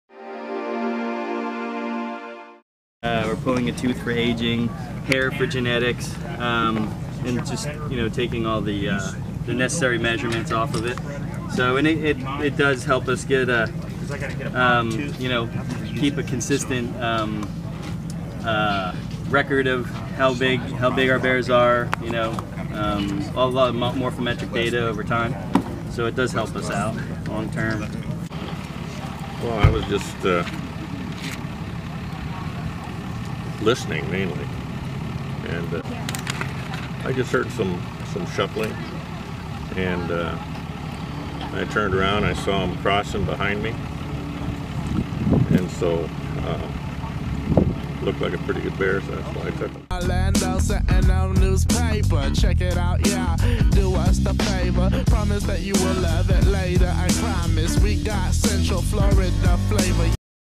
Bear Hunt Survey in Florida
Biologists are taking measurements of bears killed to get a better grasp on the population of bears around central Florida.